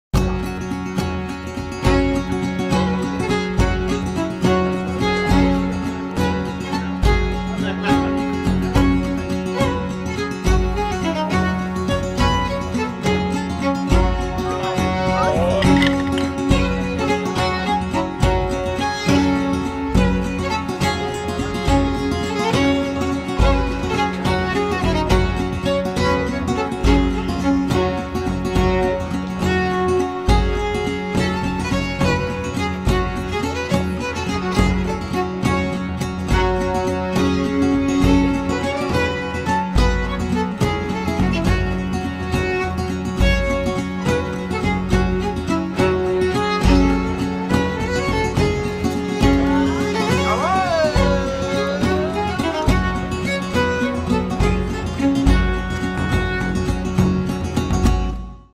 Kaltenberger Ritterturnier - Trailer Genuss und Gaumenfreuden 2018
Die Hofköche bieten wohlschmeckendes mittelalterliches Essen verschiedenster Arten an. Ob Spanferkel, Würstl, Fisch, Suppen oder orientalische Früchte – hier bleibt kein kulinarischer Wunsch offen und keine Kehle trocken. Köstlicher Met und das exzellente Dunkelbier aus der Königlichen Schlossbrauerei zu Kaltenberg erfrischen nicht nur; sie geben auch Kraft für die nächsten Entdeckungstouren. Und diese kann bis in den späten Abend dauern  – so viel gibt es auf dem Schlossgelände zu erleben.